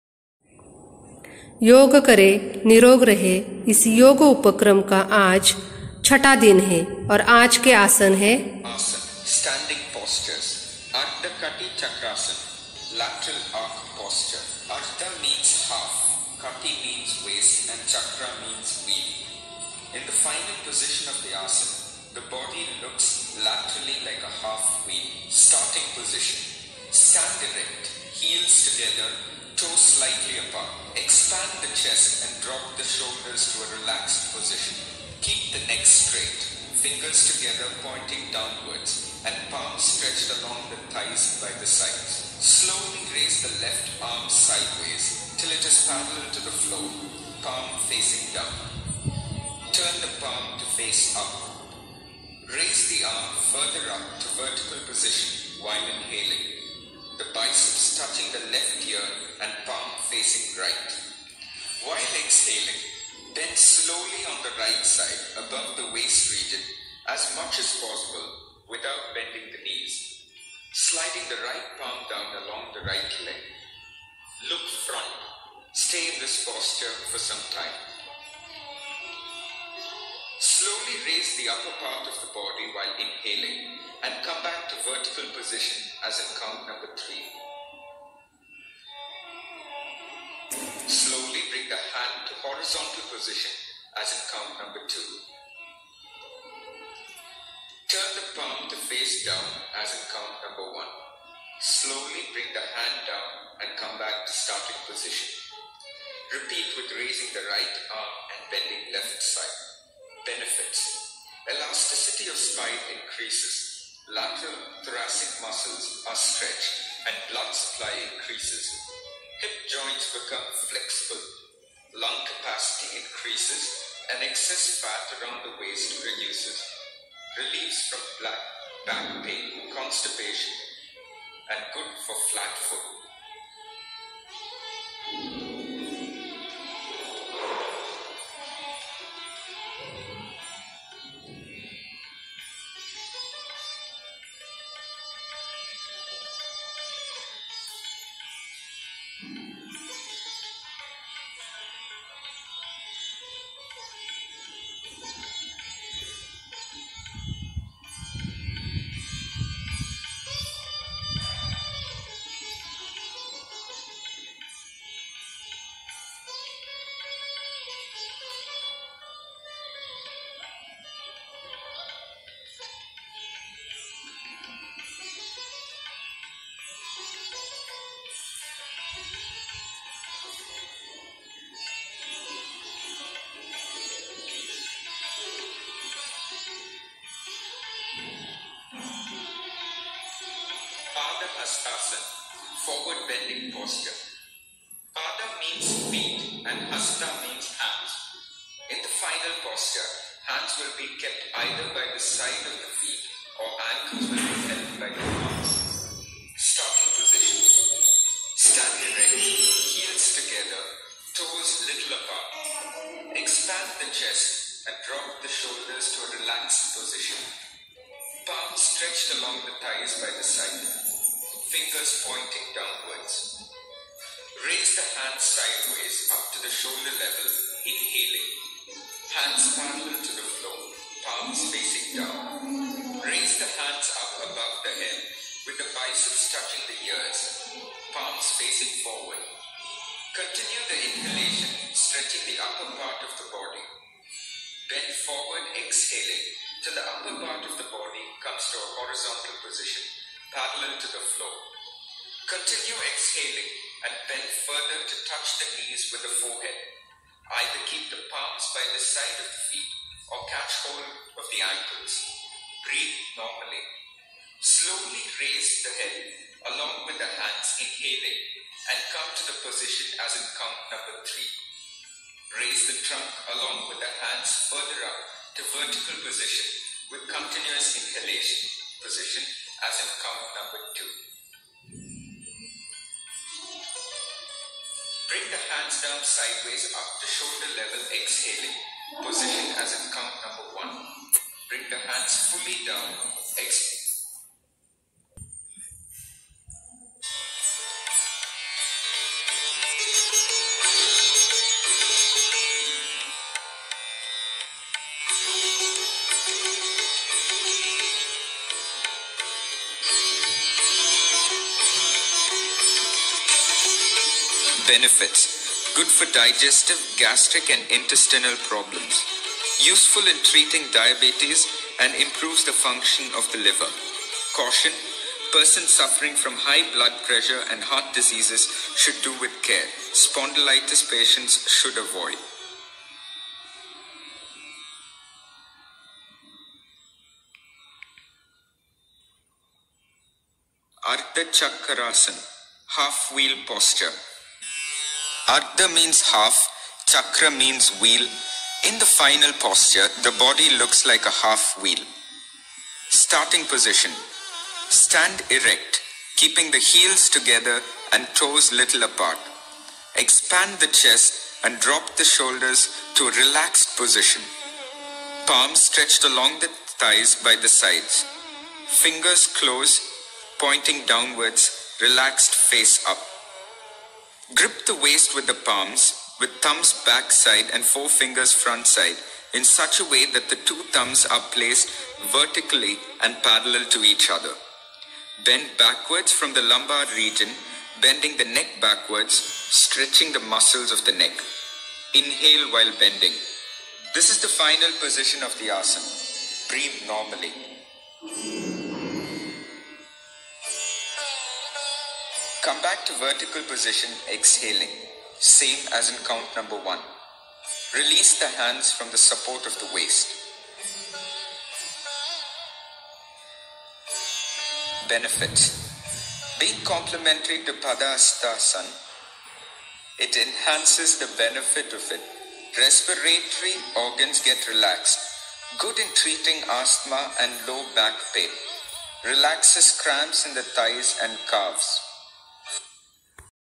Yoga workshop day 6
Ardhakati chkrasan, Padhastasan, Ardhachakrasan